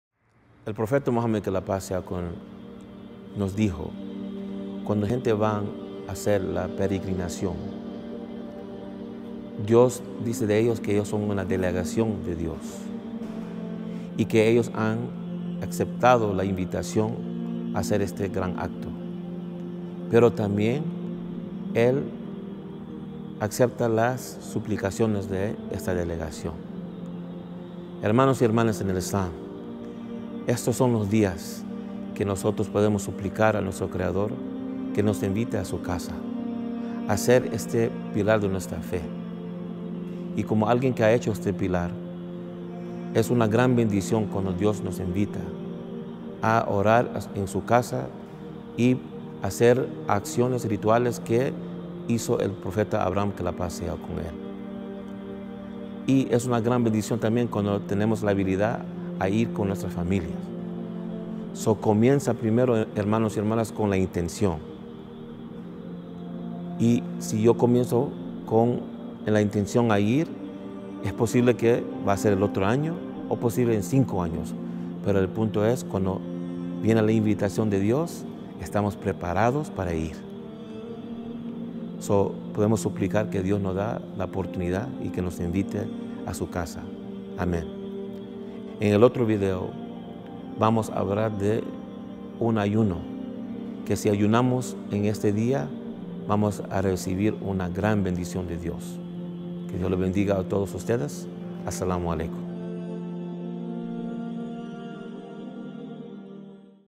Consejo 7: "Solo por Invitación" | Los Mejores 10: Consejos para Conversos
"Cuando Allah nos llama, somos una delegación de Allah". Oramos para que Allah nos invite a convertirnos en sus invitados a su casa en el futuro cercano, Ameen.

Los consejos de hoy nos recuerdan que primero debemos tomar la intención de visitar un día la casa de Allah y estar listos para recibir Sus bendiciones al aceptar la invitación.

Mañana veremos las bendiciones del ayuno en el día especial de Arafah. Comparta este video con todos sus seres queridos para que ellos también puedan maximizar sus buenas acciones en estos días bendecidos.

*Our Mission: To Inspire, Lead, and Pioneer Solutions In Educating Latinos and Other Communities About Islam Through Media Productions and Grassroots Outreach Worldwide.